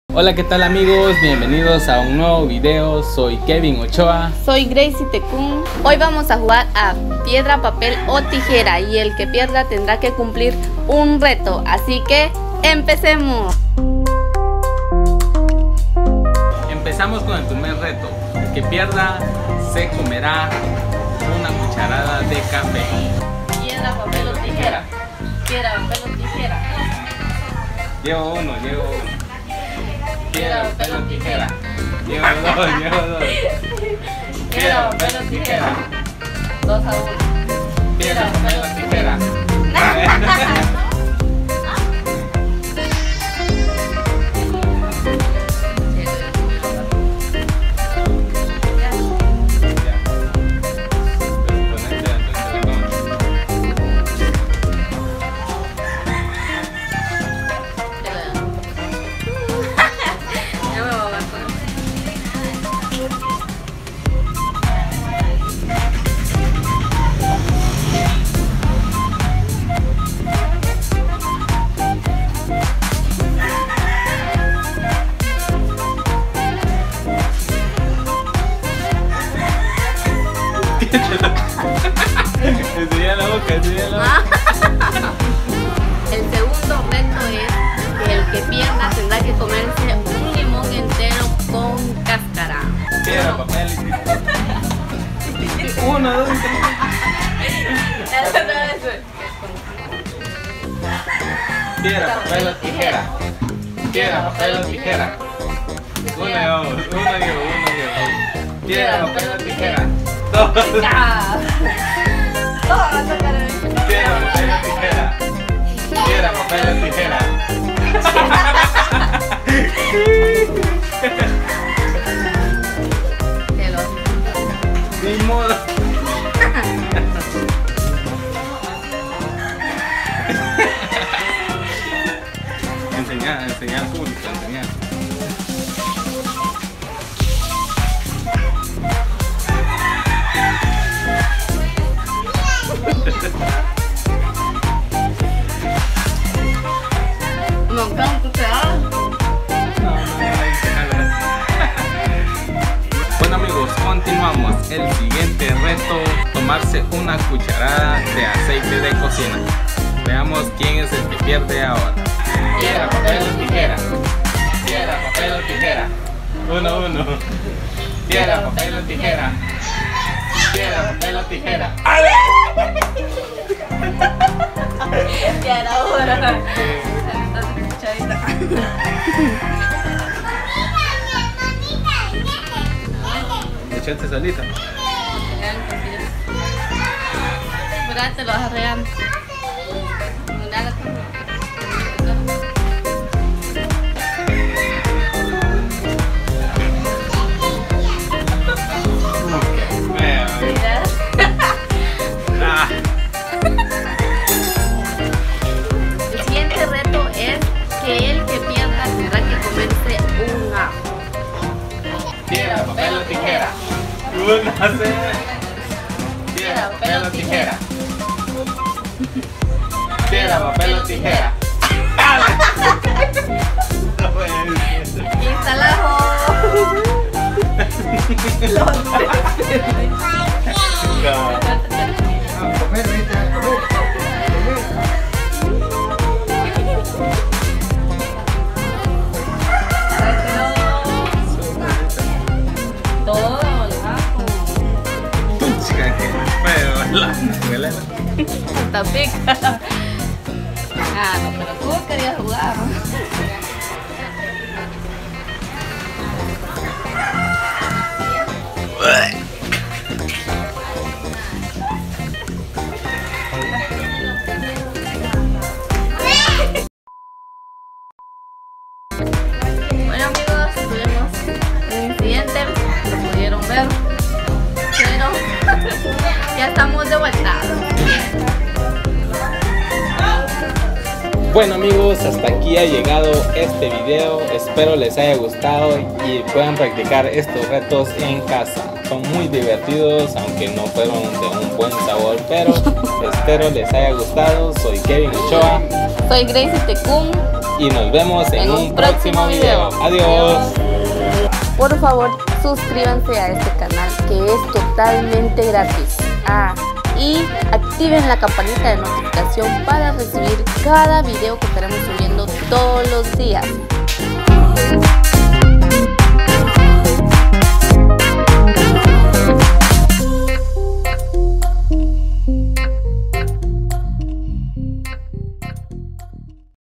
PIEDRA, PAPEL O TIJERA - Vídeos Chapines Y Mas
🙏💥Hola amigos!!!💥🙏

Como están?? Hoy les traemos un juego muy conocido como "piedra papel o tijeras" es muy divertido para distraernos y convivir con nuestros familiares en esta cuarentena, ya que lo puedes convinar con diferentes retos y hacerlo aún más divertido, aquí les mostraremos como lo aremos nosotros😄👍

Espero les guste el video amigos!

Porfavor regalanos tu SUSCRIPCIÓN!!!👍🙏
Hasta pronto amigos!!